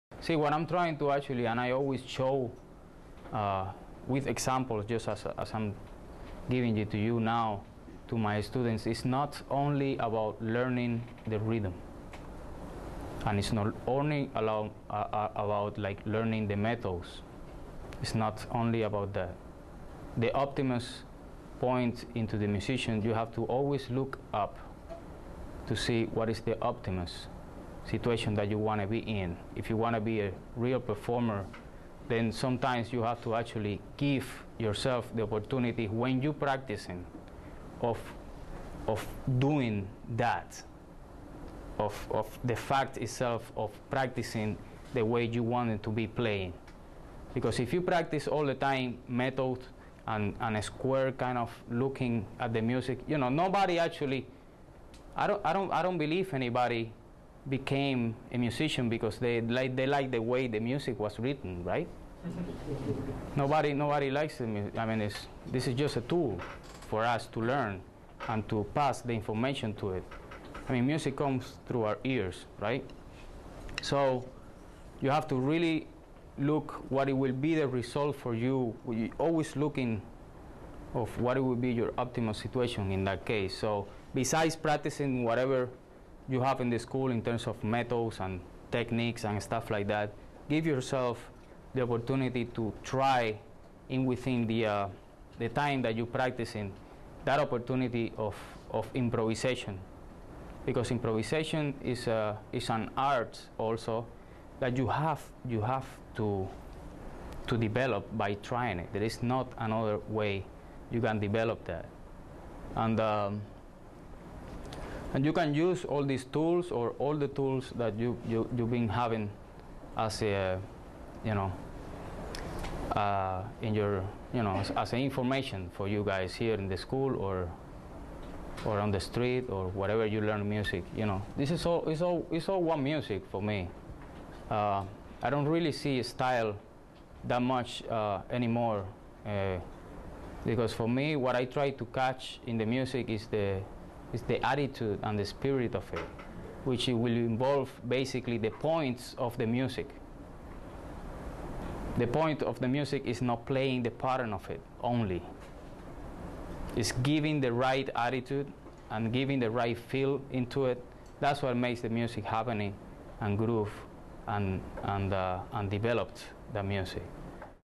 Master Class: Latin Percussion Technique, Part 4: Practicing Rhythms,Musicianship
- Drummer and professor of Percussion Dafnis Prieto discusses his approach to the mental side of practicing, and why he feels that what written on the page should not always be the main focus of your practice.